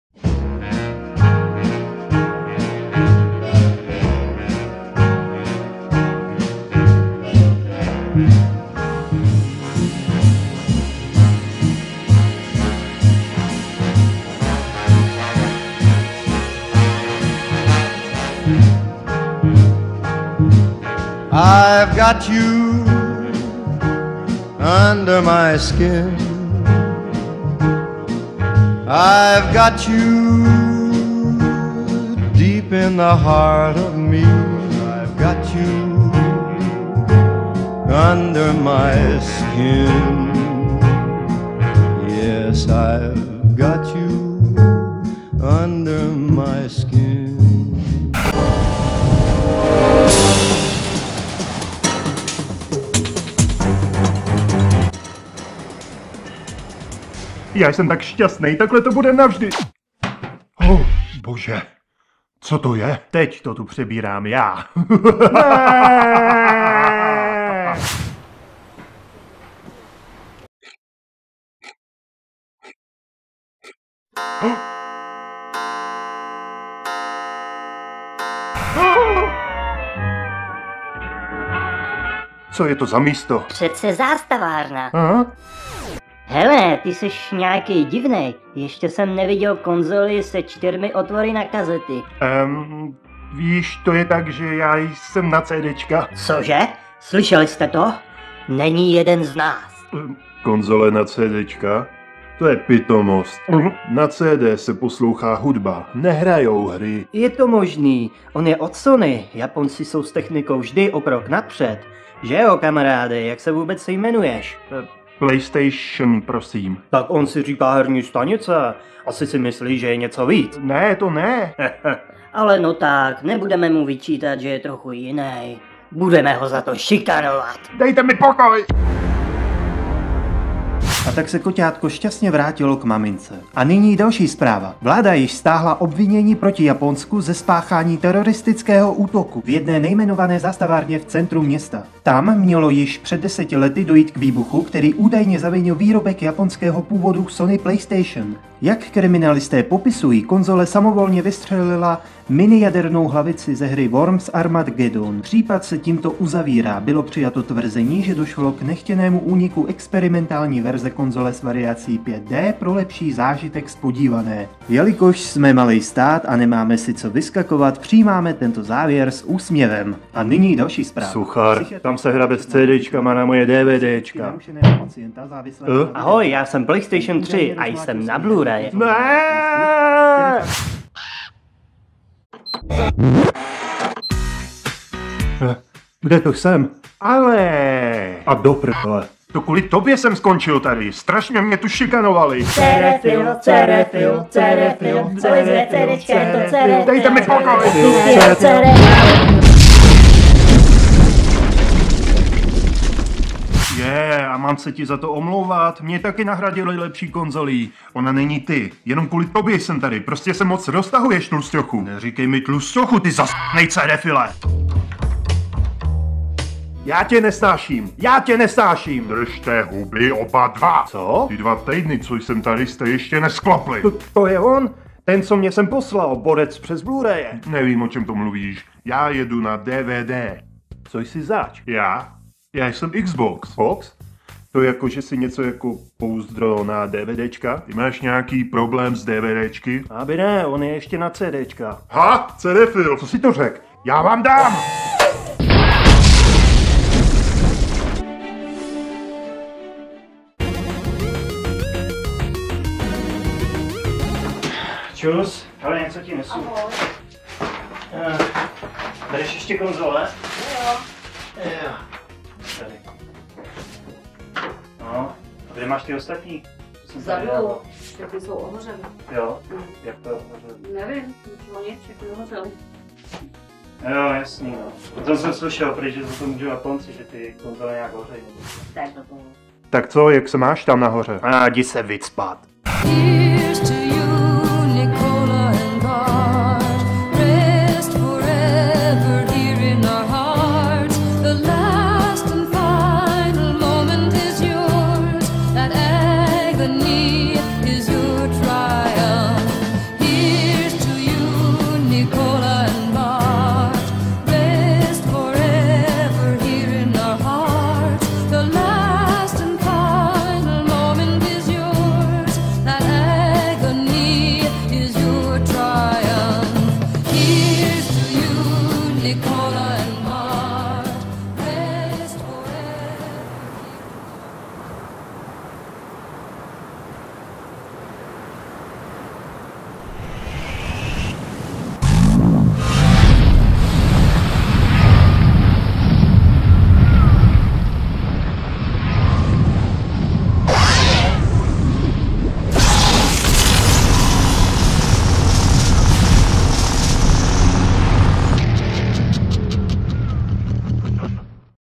PlayStation 3 vs XBOX 360 Origins .08 ( CZ parody )
Tak se podíváme trochu nazpátek jak se mám konzole potkaly a co předcházelo nesváru mezi nimi . Je tu nový díl o konzolích , bavte se :)